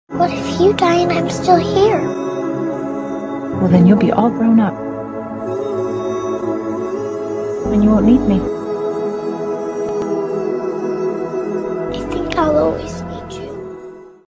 I think I’ll always need you 🥺💕 (CW: Mild shake, glitch, flash?)
Have some Kaiya and Tatsu (sort of) angst 🥲

I found this audio in a Danganronpa TikTok compilation and thought it would fit 🙃

THEM 😢💞